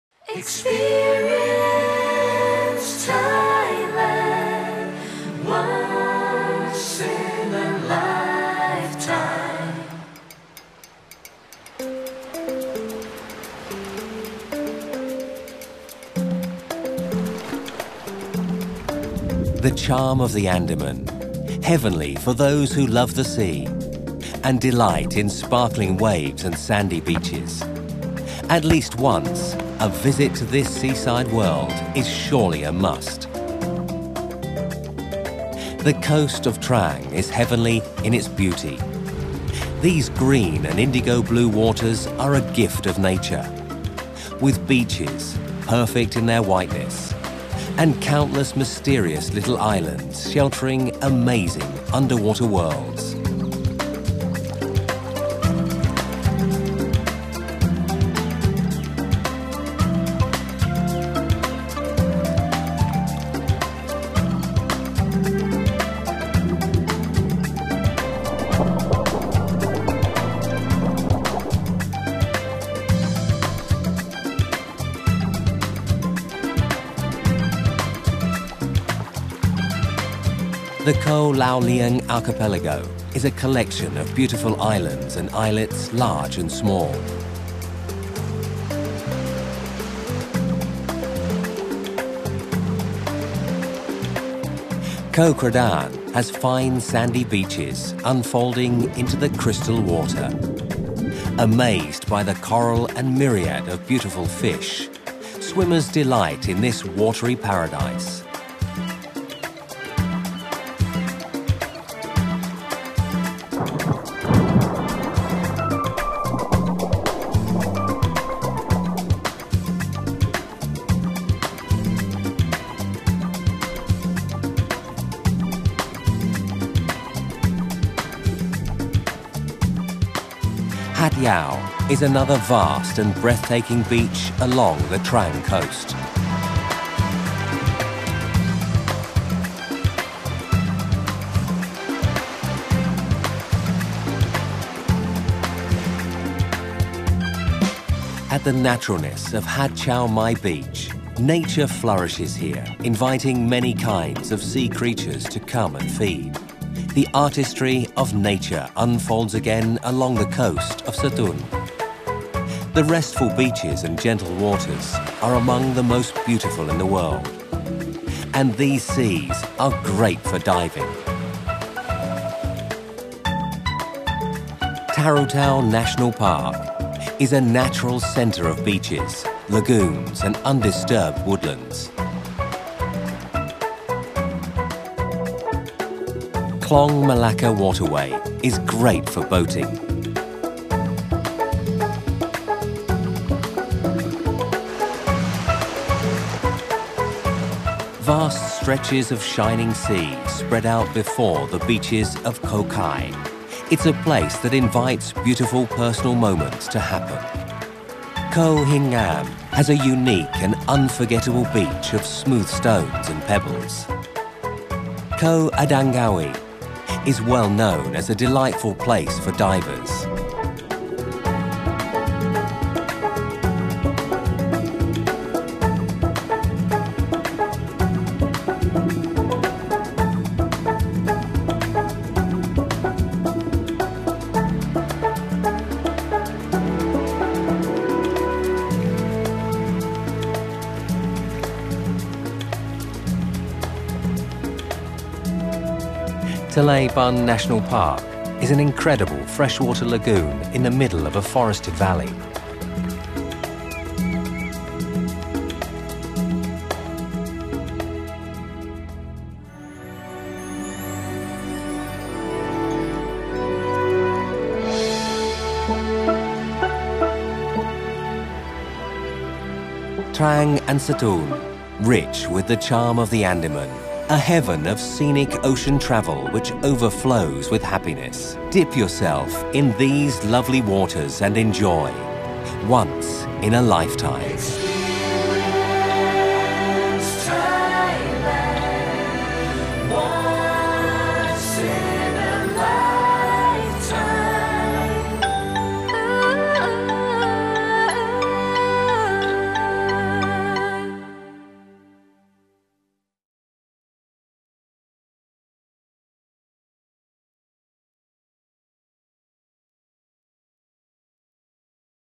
トラン＆サトゥン
マレー語で「夜明け」を意味するトラン。その昔貿易港として暁の中を船が行き交ったいにしえの余韻漂う美しい名のごとく、県土の3分の２が自然公園に指定されたトランは無垢の自然が残るナチュラルリゾートとして羨望を集めはじめています。